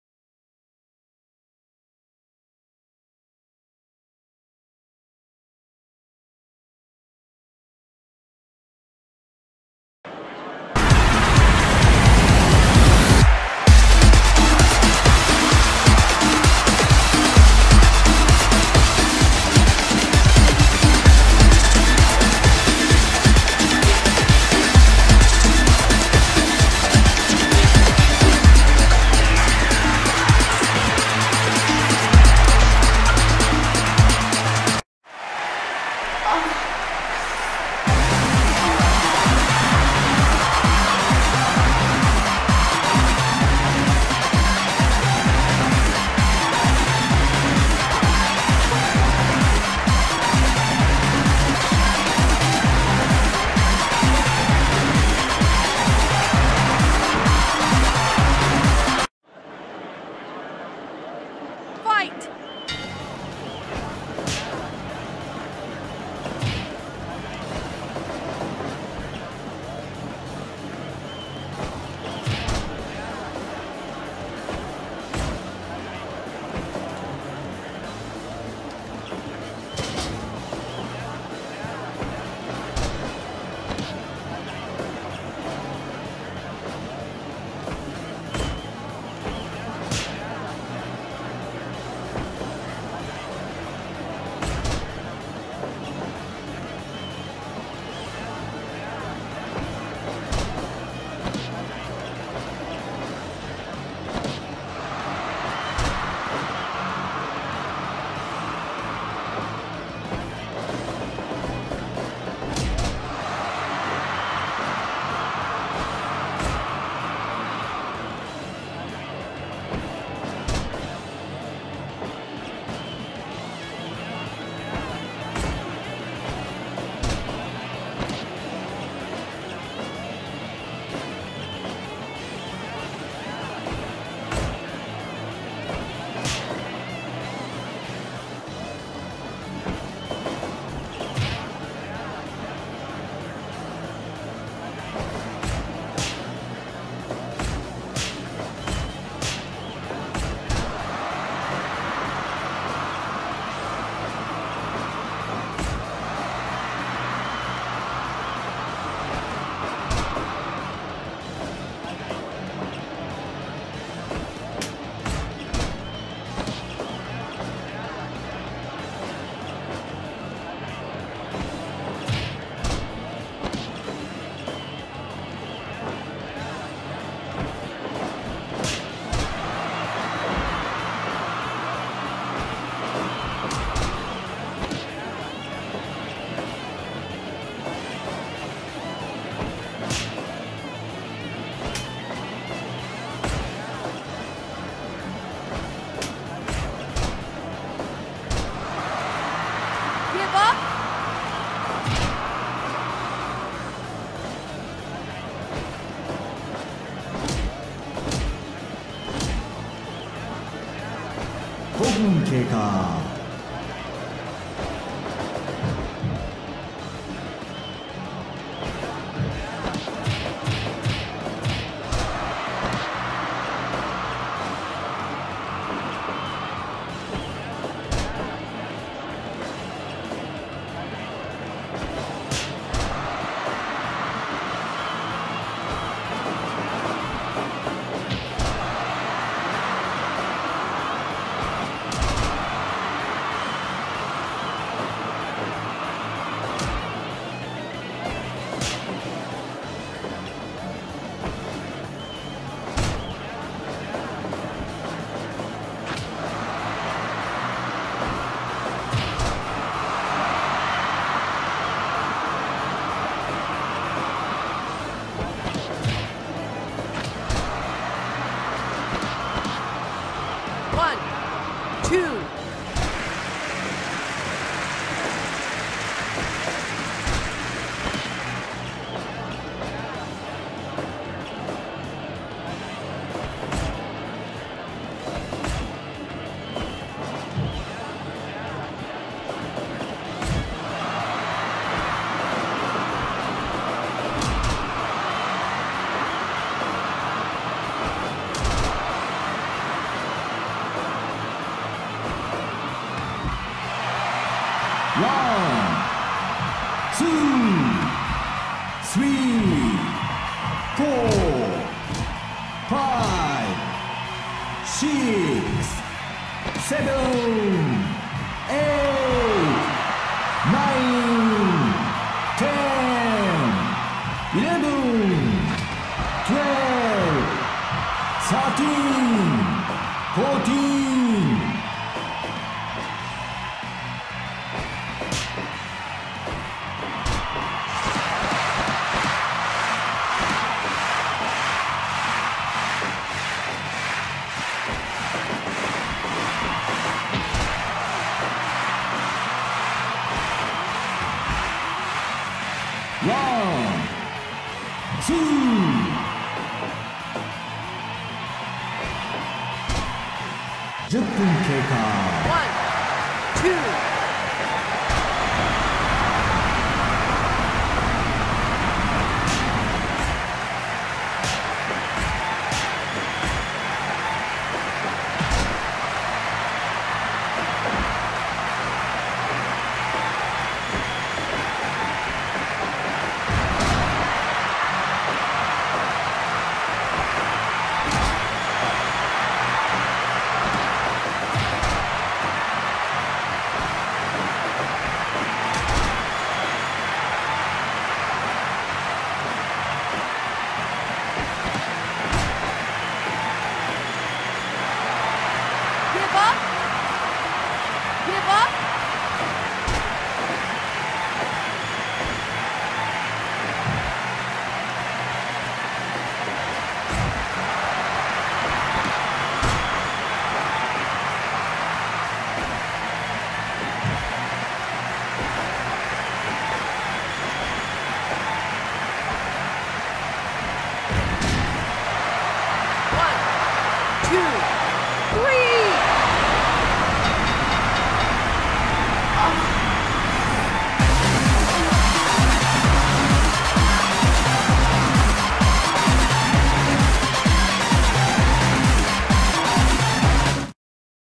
FPCW HeatWave: Eighth Bout: Hana Kimura Cup: Tay Melo v. Edith Surreal
The next match in the Hana Kimura Cup, as Tay Melo faces off against the Walking Masterpiece, Edith Surreal!